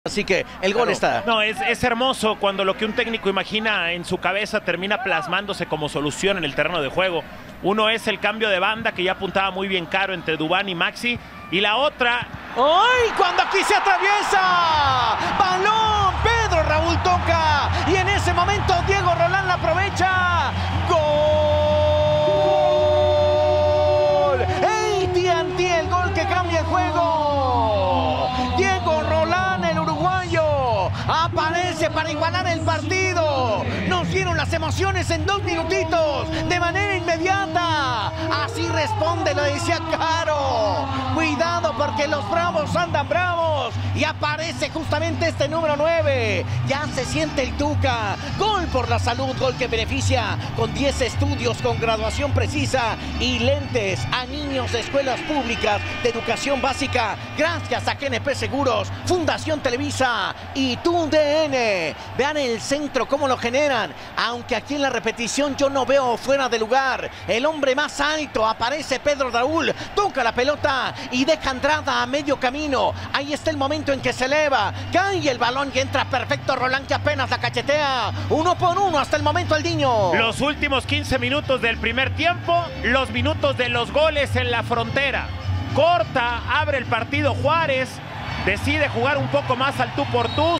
¡RESPONDEN! Gol de Diego Rolan | FC Juárez 1-1 Rayados | Grita México BBVA AP2021 - J11 | TUDN
Juárez responde con un gran gol de Diego Rolan y empata el juego.